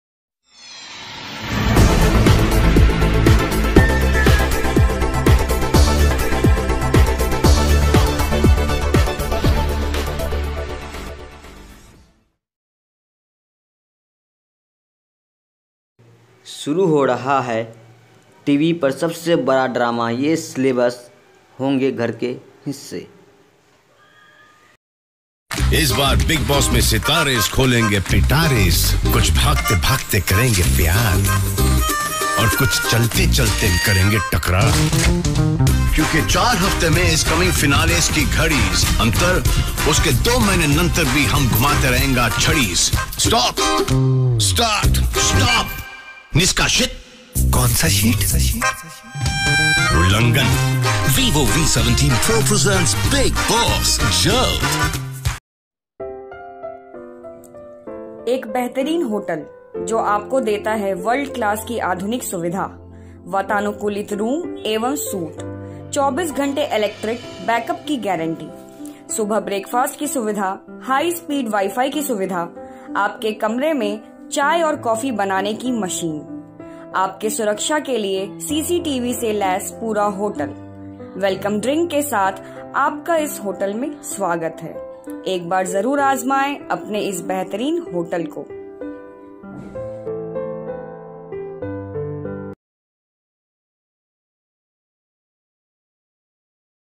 आज से शुरू होगा टीवी पर सबसे बड़ा ड्रामा
आज से शुरू होगा टीवी पर सबसे बड़ा ड्रामा, ये सेलेब्स होंगे घर का हिस्सा!

रविवार की रात से टीवी जगत का सबसे बड़ा 'ड्रामेबाज' शो यानी बिग बॉस की शुरुआत होने वाली है। यह शो का 13वां सीजन होगा, जो कि हर बार से ज्यादा मजेदार होगा। बताया जा रहा है कि इस बार शो के शुरुआत का एक महीना खास होगा और लोगों को बड़ा सरप्राइज मिलेगा। शो के होस्ट सलमान खान भी यह बात कह चुके हैं कि इस बार के शो में पहले 30 दिन खास होंगे।